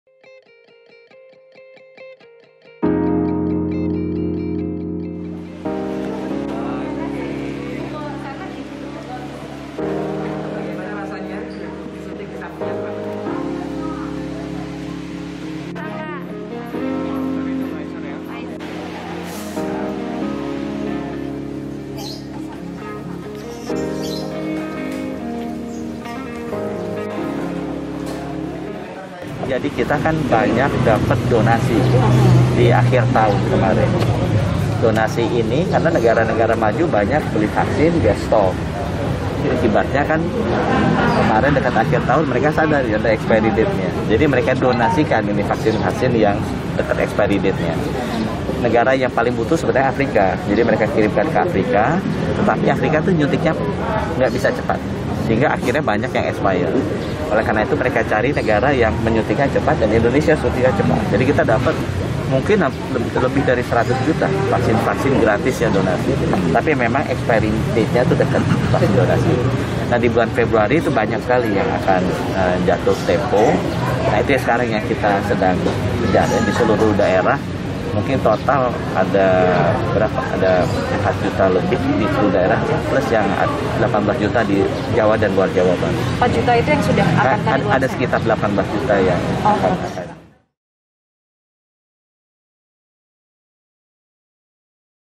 18 Juta Vaksin Donasi dari Negara Maju Expired Akhir Februari
JAKARTA, KOMPAS.TV - Menteri Kesehatan RI Budi Gunadi Sadikin menjelaskan sebanyak 18 juta vaksin gratis akan jatuh tempo atau expired pada akhir Februari 2022.

Guna mengantisipasi hal itu, vaksin booster disejumlah wilayah terus dilakukan agar vaksin yang berasal dari donasi negara maju dapat digunakan sebelum jatuh tempo.

Hal itu disampaikan Menteri Kesehatan Republik Indonesia Budi Gunadi Sadikin saat meninjau jalannya vaksinasi booster di Balai Sarwono, Cilandak Timur, Jakarta Selatan.

Baca Juga 110.000 Vaksin di Bengkulu Akan Kadaluarsa 2 Pekan Lagi, Ini Penyebabnya... di

Budi Gunadi menjelaskan negara Indonesia yang mendapatkan donasi vaksin dari negara negara maju vaksin yang didonasikan itu jatuh tempo atau expirednya dalam waktu dekat.